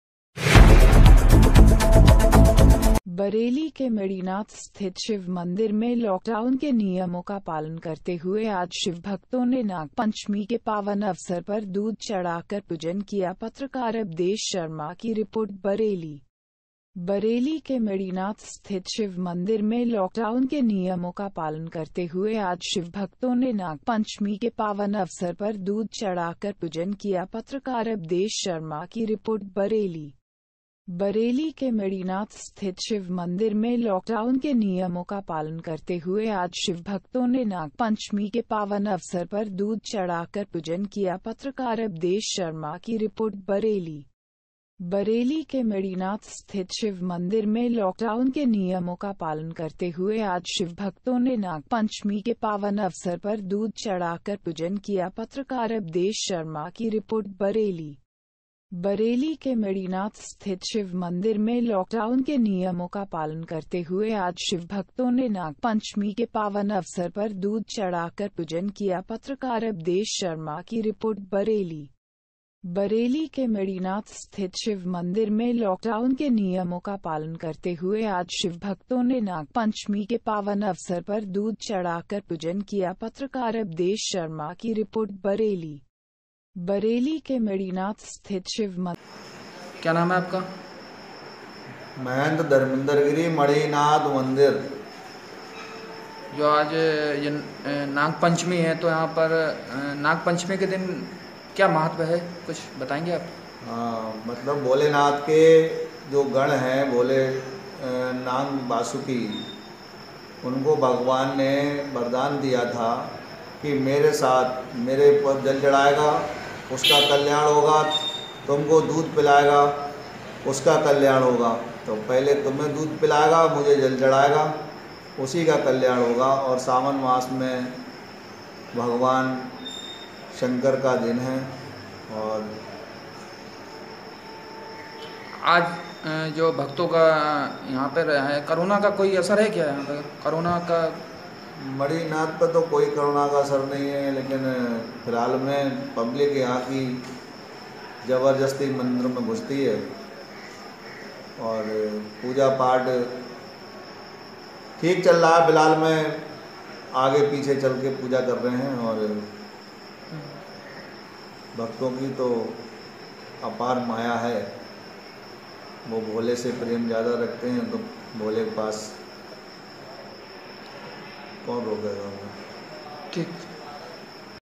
आज बरेली के मढ़ीनाथ स्थित शिव मंदिर पर भक्तों ने नाग पंचमी के पावन अवसर पर दूध चढ़ाकर पूजन किया
पत्रकार अबदेश शर्मा की रिपोर्ट बरेली